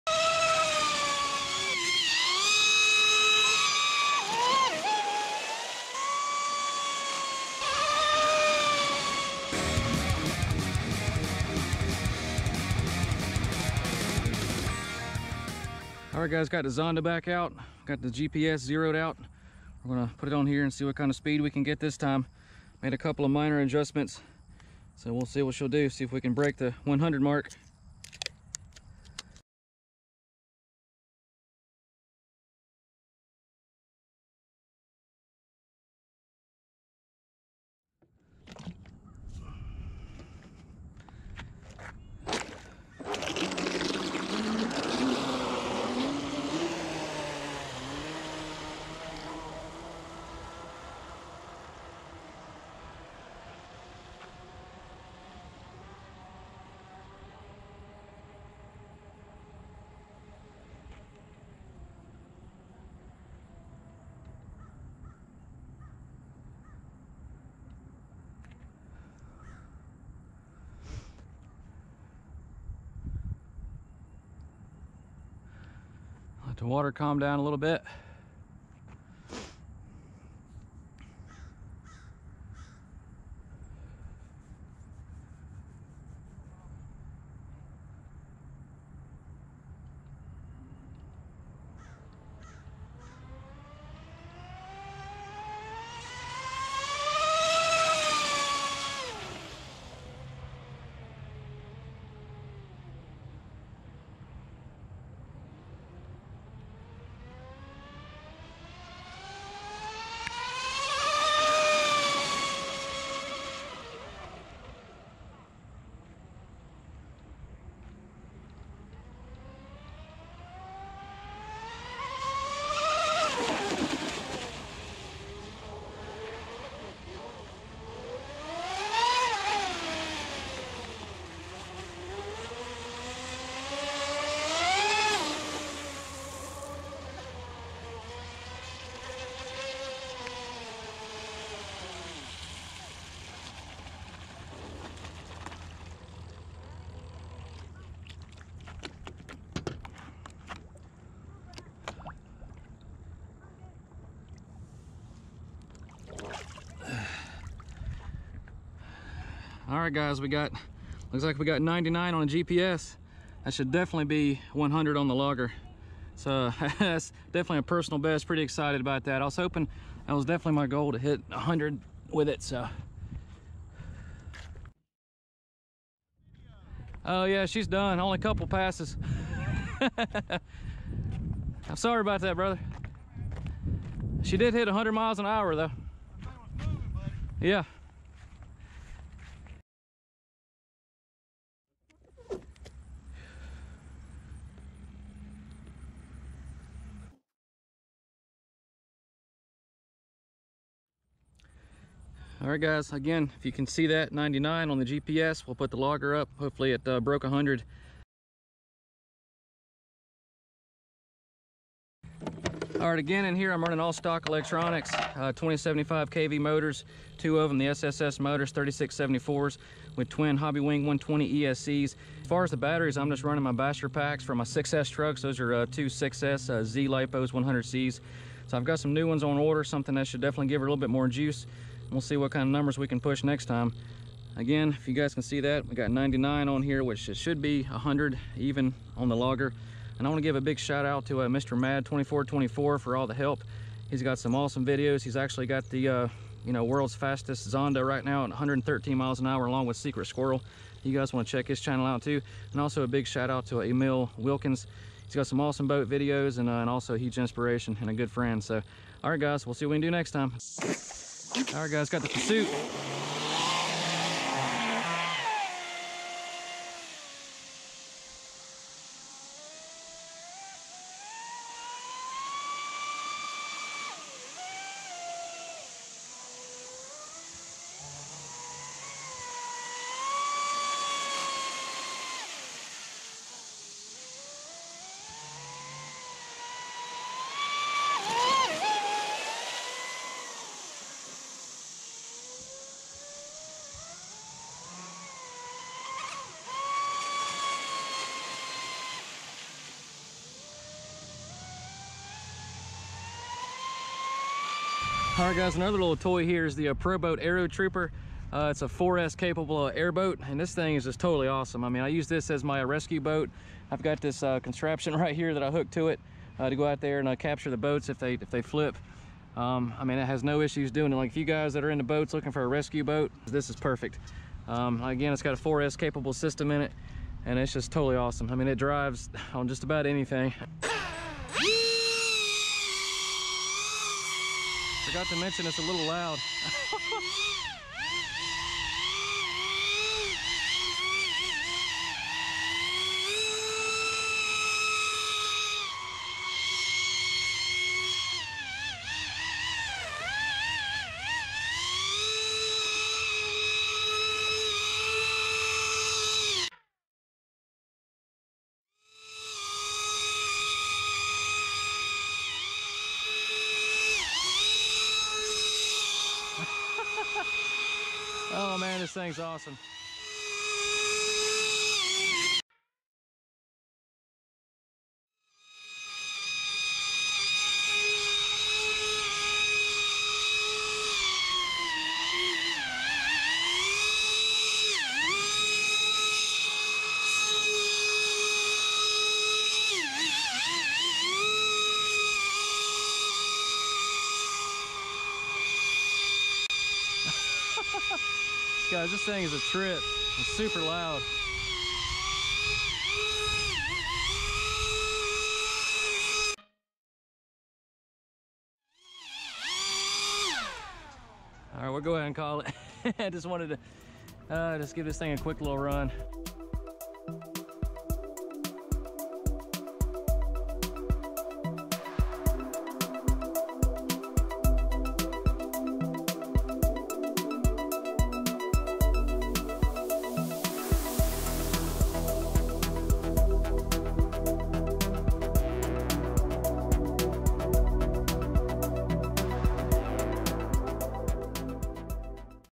TFL ZONDA 101mph! -with TFL Pursuit and Pro Boat Aerotrooper rip!
In the quest for 100mph and finally did it! Got the TFL Zonda out for another speed run and hit 101 by GPS datalogger. Running stock electronics with Dasboata props. I have some new batteries coming to hopefully push it harder next time! Also added footage of the TFL Pursuit and Pro boat Aerotrooper Air boat!